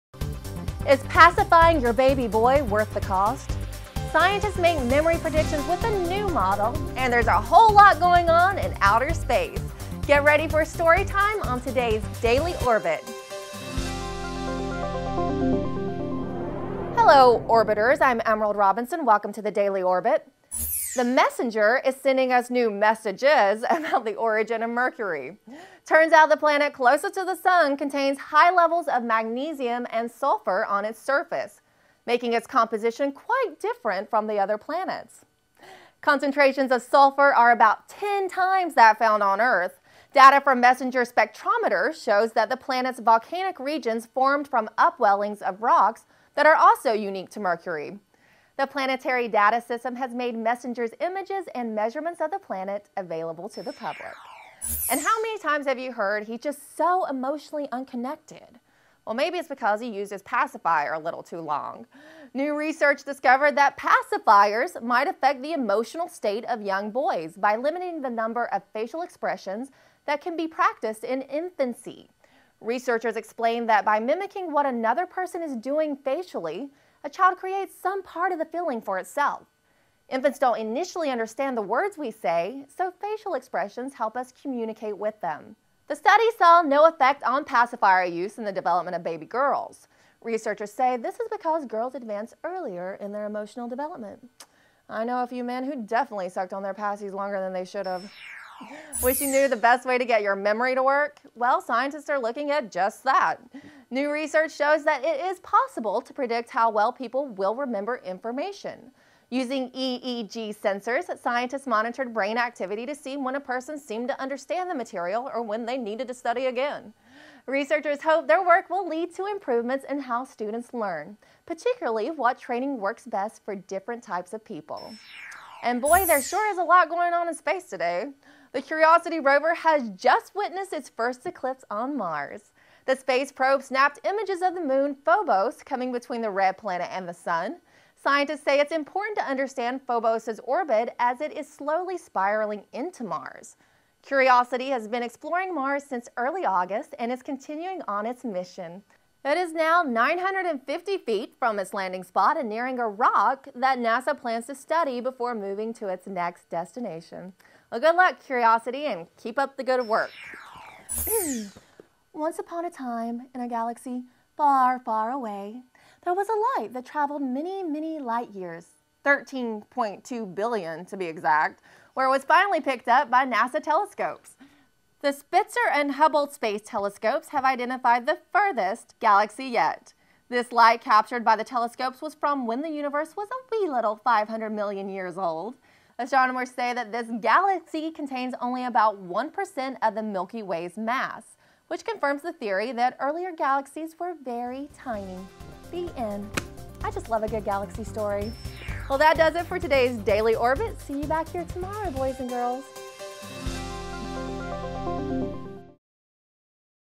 Baby Pacifiers Not Good For Boys - The Daily Orbit
Is the pacifier putting a lid on boy's emotional growth?

Scientists make memory predictions with a new model.

And whole lot of space time!

Get ready for story time on today's Daily Orbit!

Hello Orbiters, I'm Emerald Robinson. welcome to the Daily Orbit!

The Messenger is sending us new messages about the origin of Mercury. Turns out the planet closest to the sun contains high levels of magnesium and sulfur on its surface, making its composition quite different from the other planets.  Concentrations of sulfur are about 10 times that found on Earth.  Data from Messenger's spectrometer show that the planet's volcanic regions formed from upwellings of rocks that are also unique to Mercury.  The Planetary Data System have made Messenger's images and measurements of the planet available to the public.

How many times have you heard: "he's just so emotionally unconnected!" Well, maybe it's because he used his pacifier a little too long.  New research discovered that pacifiers might affect the emotional state of young boys by limiting the number of facial expressions that can be practiced in infancy.  Researchers explain that by mimicking what another person is doing facially, a child creates some part of the feeling for itself.  Infants don't initially understand the words we say, so facial expressions help us communicate with them.  The study saw no effect on pacifier use in the development of baby girls.  Researchers say this is because girls advance earlier in their emotional development.  I know a few men who definitely sucked on their "passies" longer than they should have!

Wish you knew the best way to get your memory to work?  Well, scientists are looking at just that!  New research shows that it is possible to predict how well people will remember information.  Using EEG sensors, scientists monitored brain activity to see when a person seemed to understand the material or when they needed to study again.  Researchers hope their work will lead to improvements in how students learn, particularly what training works best for different  types of people.

And boy there's a lot going on in space today! The Curiosity Rover has just witnessed its first eclipse on Mars.   The space probe snapped images of the moon Phobos coming between the Red Planet and the Sun.   Scientists say it's important to understand Phobos' orbit as it is slowly spiraling into Mars.  Curiosity has been exploring Mars since early August, and is continuing on its mission. It is now 950 ft from its landing spot and nearing a rock that NASA plans to study before moving to its next destination.  Good luck Curiosity and keep up the good work.

(Reading a book) Once upon a time in a galaxy far, far away there was a light and that light travel many, many light years, 13.2 billion to be exact, where it finally was picked up by NASA's telescopes.  The Spitzer and Hubble space telescopes have identified the furthest galaxy yet   This light captured by the telescopes was from when the universe was a wee little 500 million years old.  Astronomers say that this galaxy contains only about one percent of the Milky Way's mass, which confirms the theory that earlier galaxies were very tiny.  The End.  I just love a good galaxy story :)

That does it for today's Daily Orbit.  See you tomorrow boys and girls!